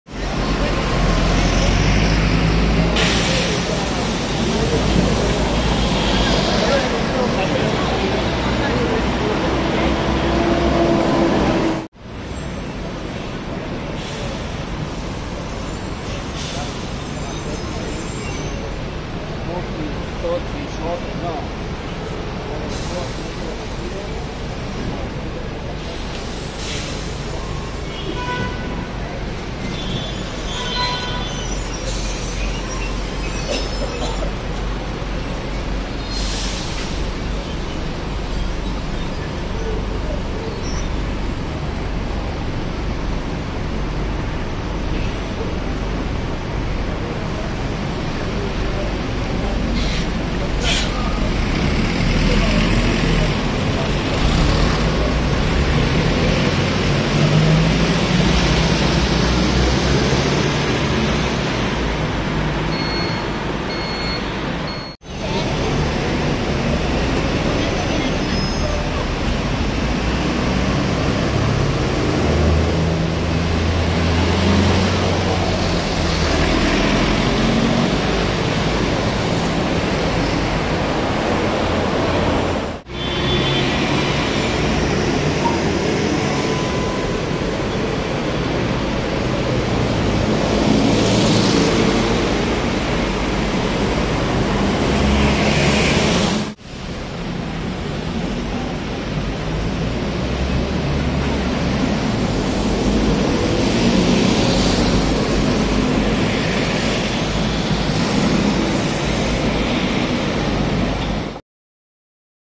[Colectivo | Línea 152] Internos 109, 78, 33, 26 y 72 - Av. Santa Fé entre Julián Álvarez y Bulnes
Filmado el Martes 04/09/2018 entre las 07:27pm y las 07:36pm.

Dispositivos utilizados:
- Zhiyun Smooth Q - Firmware v1.6.
- OnePlus 5 - Build A5000_23_180818.

Editado en Sony Vegas Pro 13.0